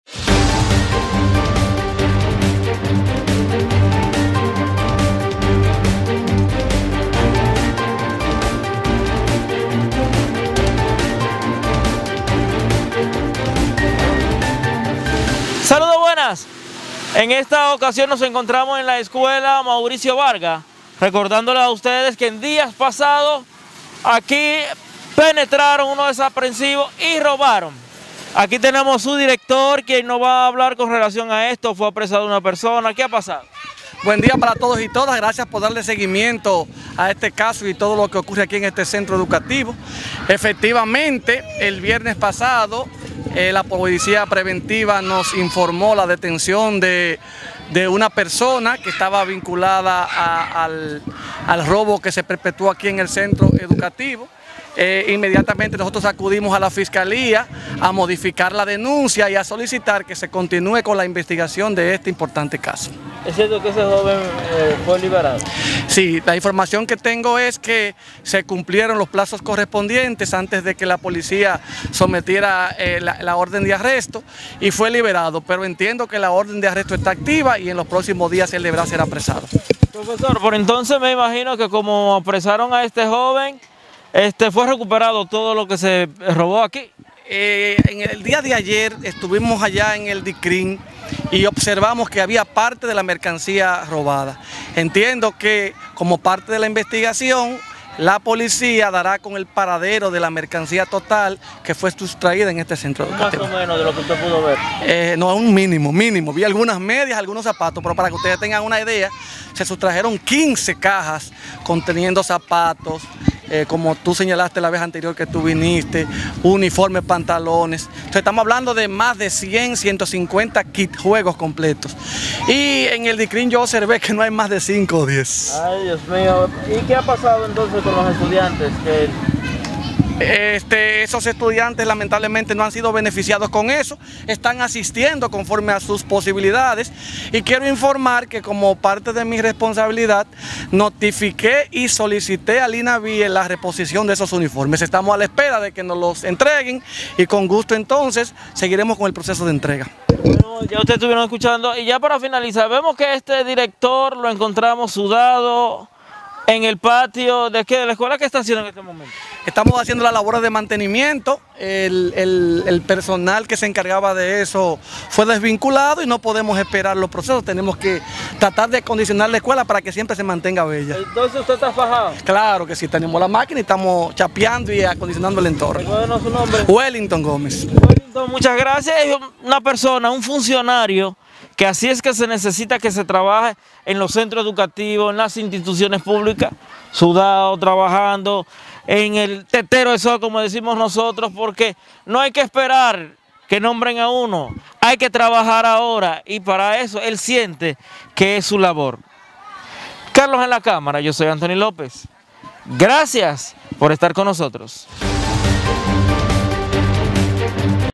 DIR. ESCUELA MAURICIO VARGAS HABLA DE INVESTIGACIÓN ROBO EN LA ESCUELA
La Vega.-El director de la escuela Mauricio Vargas. Wellington Gómez, habló sobre la investigación del robo perpetrado hace varias semanas en dicho plantel.

Gómez dijo que la policía apresó a uno de los implicados en el caso, pero que luego tuvo que ser liberado porque se venció el plazo para acusarlo.

Dijo también que ellos como centro procedieron a modificar la denuncia para que pueda continuar la investigación.

De los 150 kits sustraídos en la oficina del DICRIM se encuentran unos cinco o 10 que han sido recuperados.